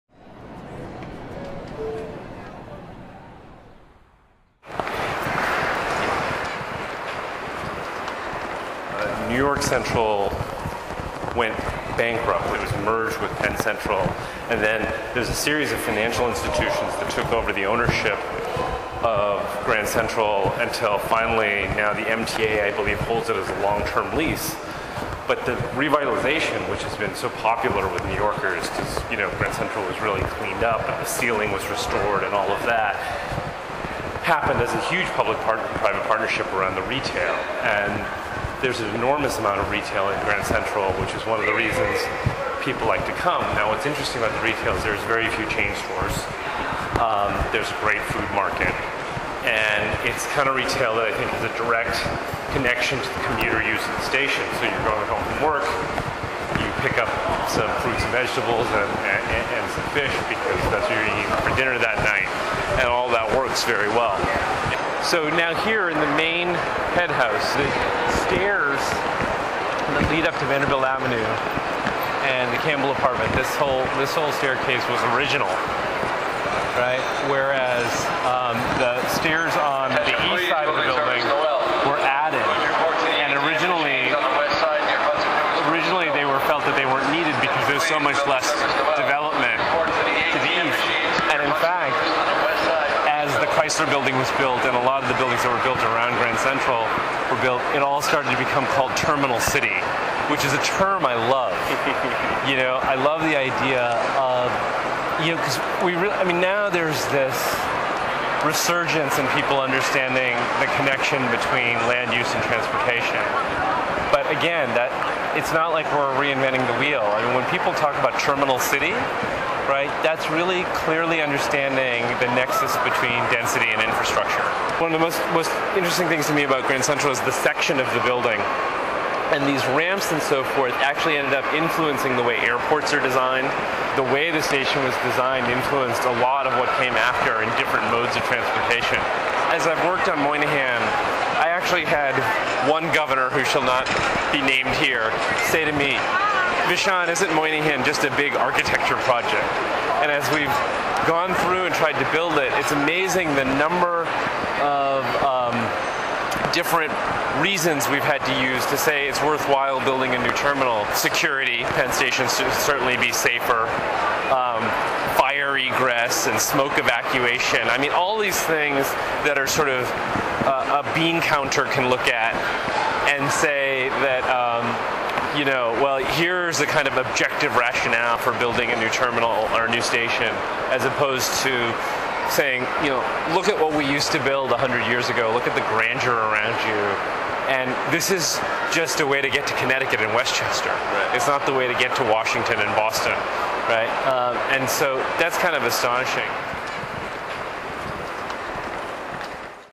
Why Grand Central Works
Urban Omnibus takes a walk with Vishaan Chakrabarti to discuss the city's favorite train station. His reflections range from design details to regional economics to the relationship between infrastructure and density.